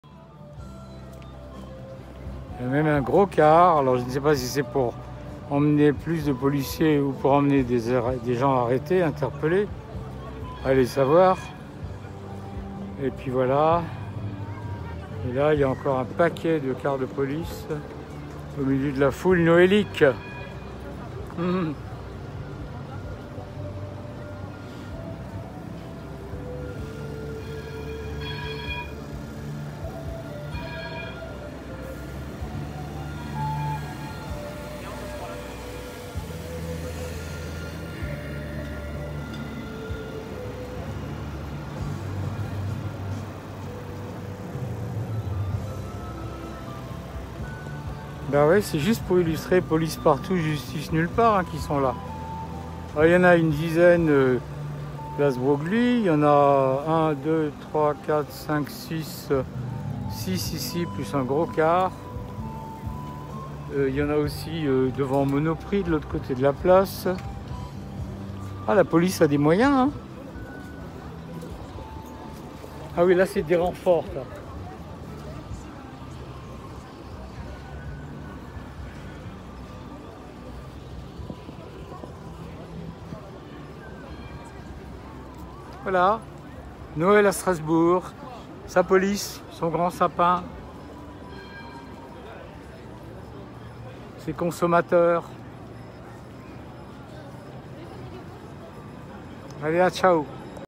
La préfète Josiane Chevalier envoie ses Robocop au centre ville la veille de Noël
Quelques personnes conspuent Macron et sa clique et voilà Strasbourg occupée...